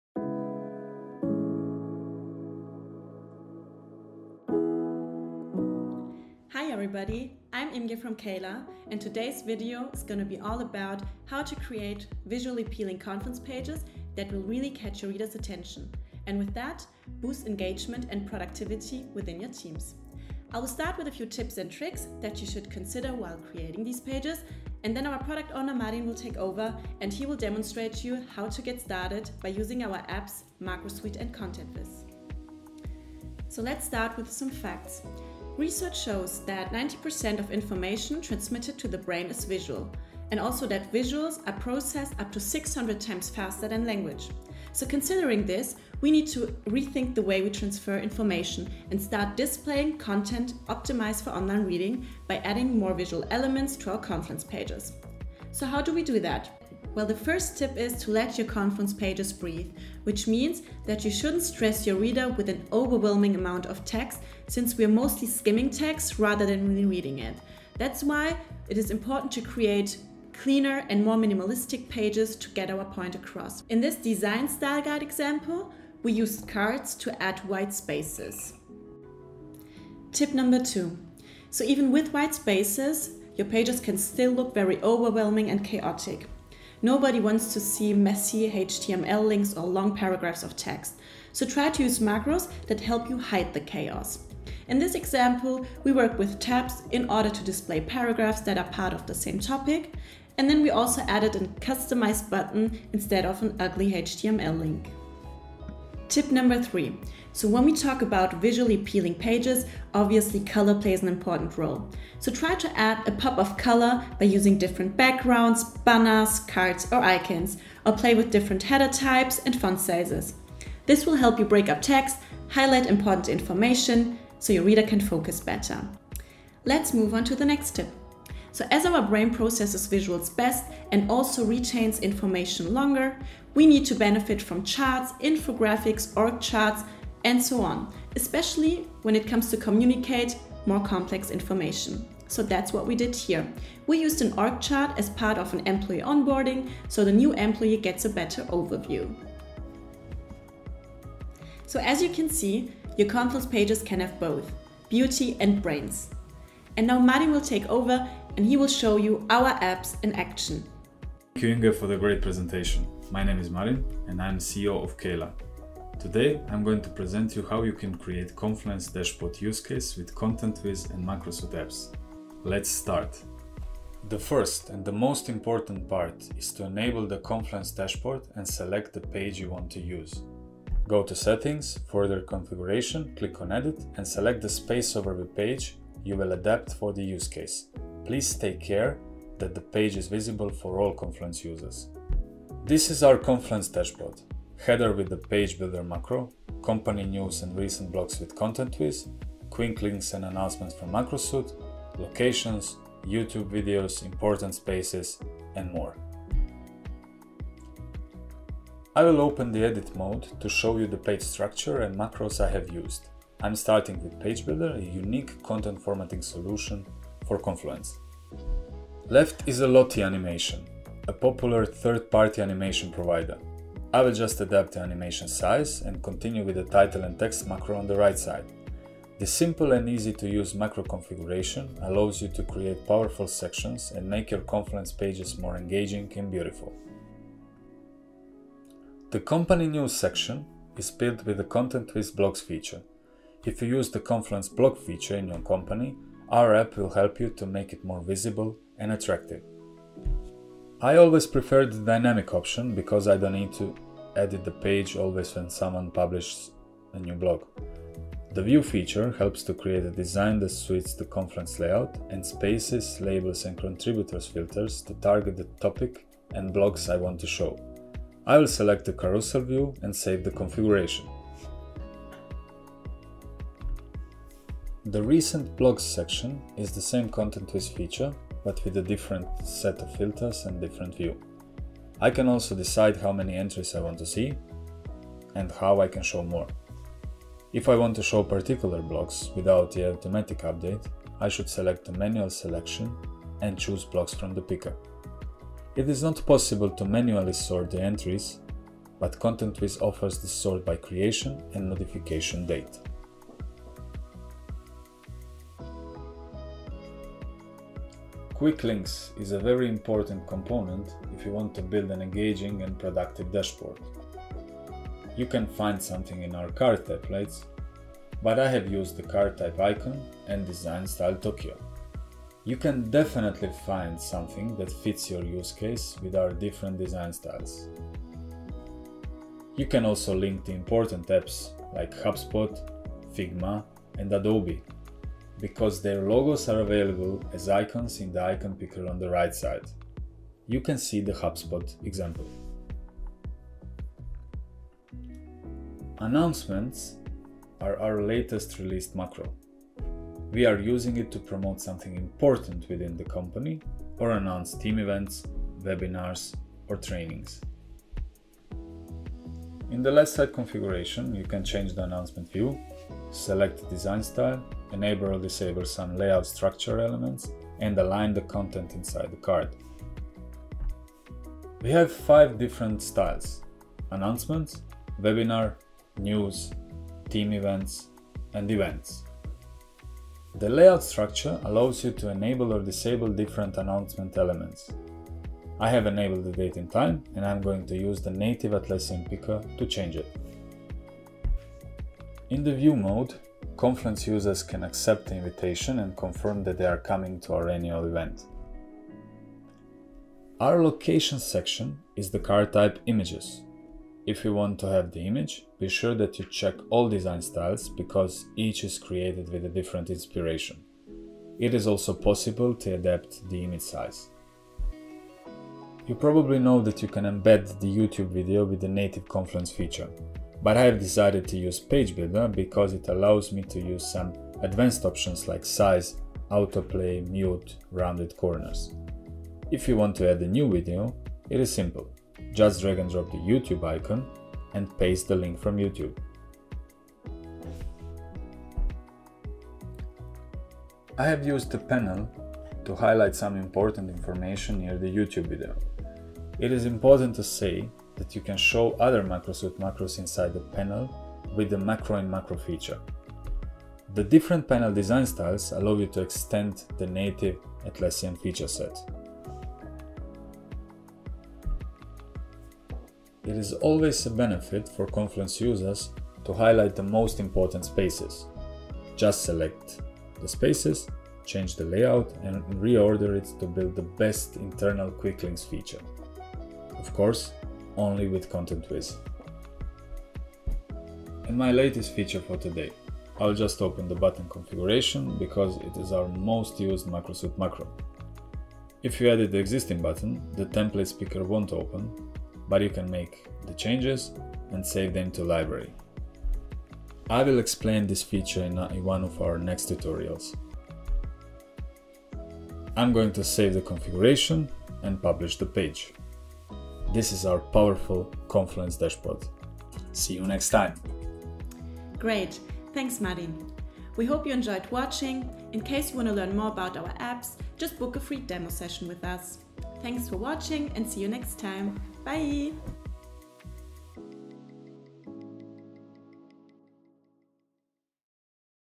How to Create Engaging Confluence Pages and Boost Collaboration
If your team isn’t using Confluence effectively, it’s not because it isn’t useful. It’s probably because it hasn’t been set up in a user-friendly way (yet!). Dumping pages and pages of information into a wiki isn’t the best way to convey information - data has to be  displayed in an engaging manner optimized for online reading. In this video, we will show you how to make your Confluence pages more engaging and which apps will help you in achieving that.